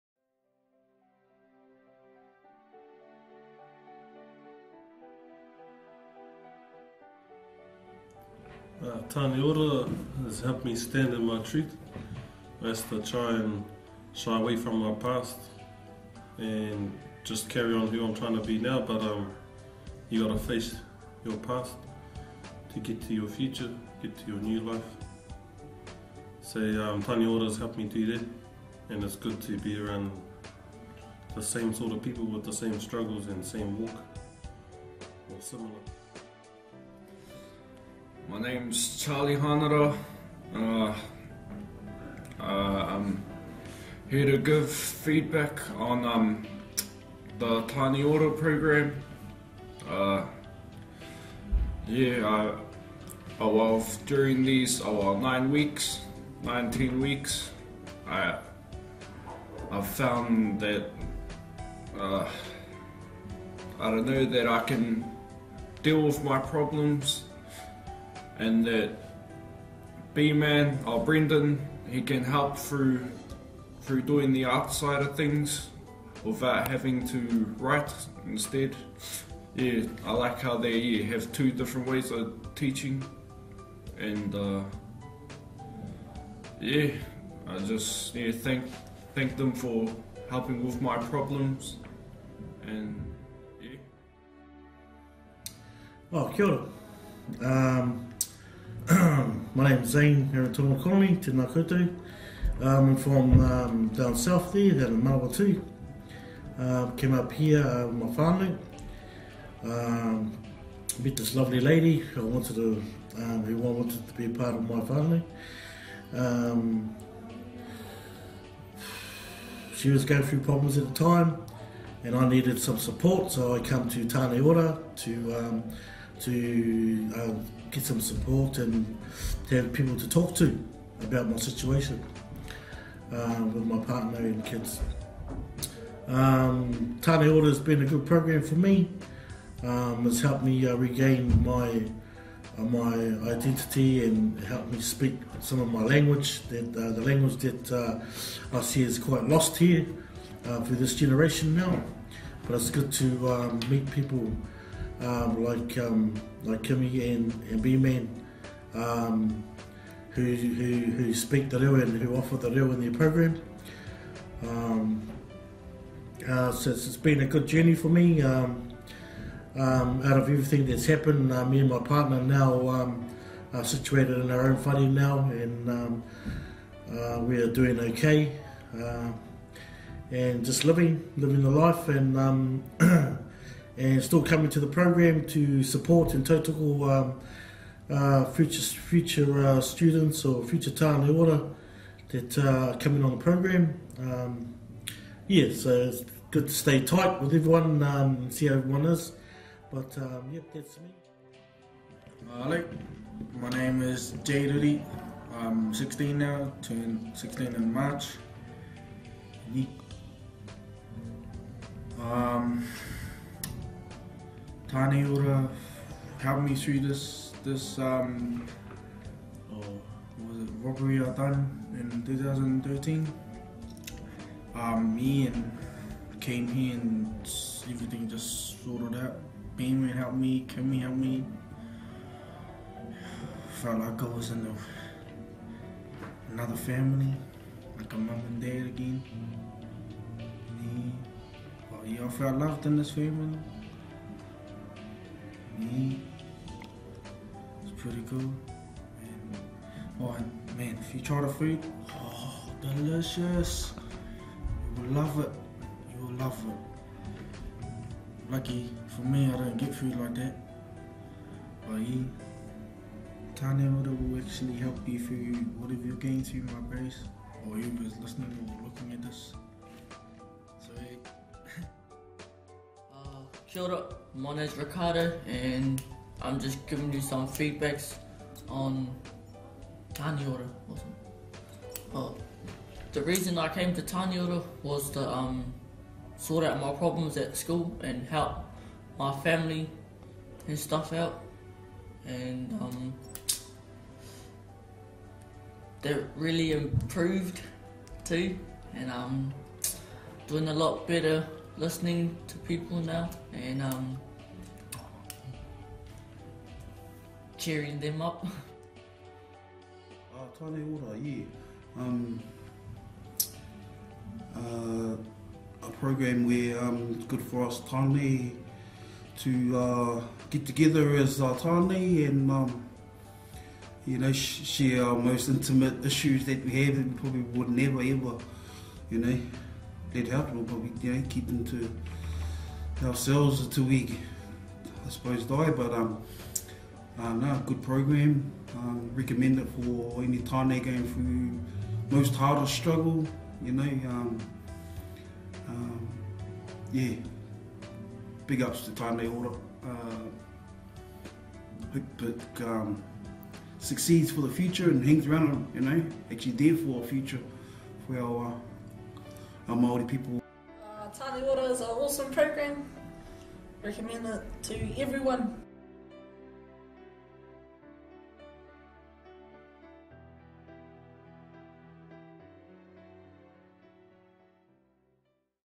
Tāne Ora Testimonials
The first testimonials gifted to us by the Tāne of Tāne Ora 2013.